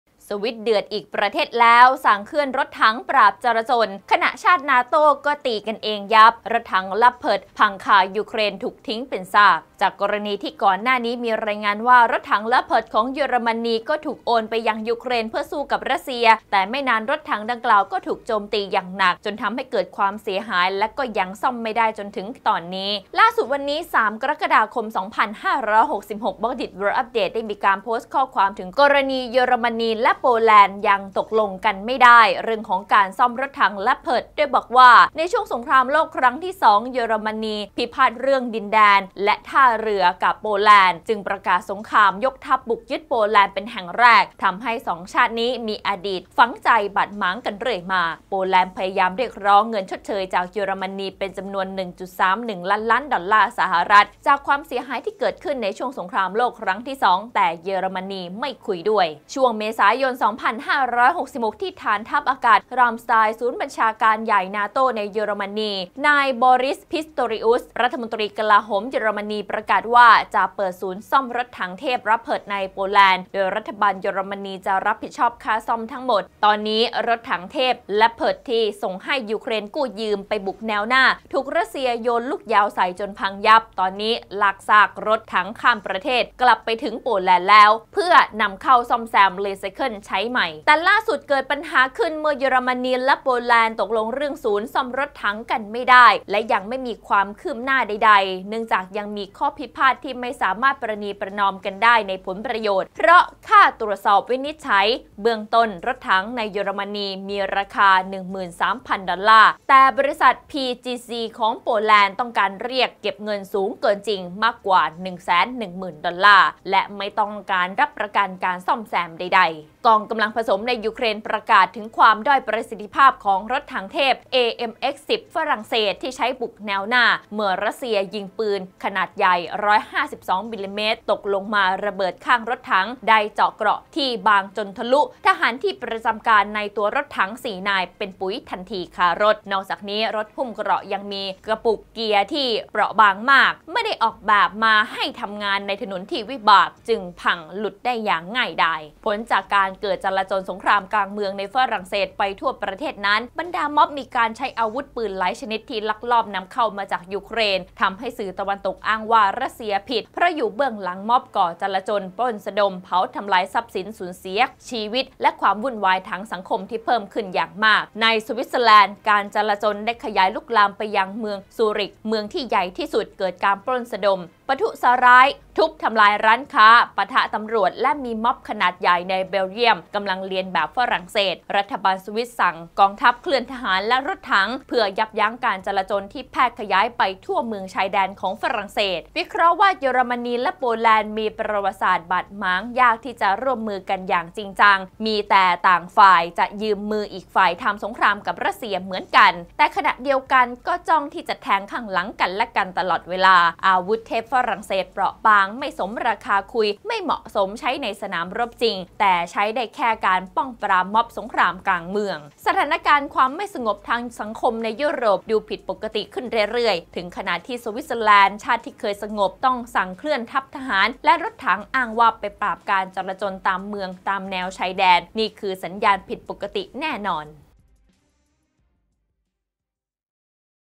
สวิสเดือดอีกปท.! เคลื่อนรถถังปราบจลาจล นาโต้ก็ตีกันเอง Leopard พังถูกทิ้งเป็นซาก
สวิสเดือดอีกปท.แล้ว! สั่งเคลื่อนรถถังปราบจลาจล ขณะชาตินาโต้ก็ตีกันเอง รถถัง Leopard พังคายูเครน ถูกทิ้งเป็นซาก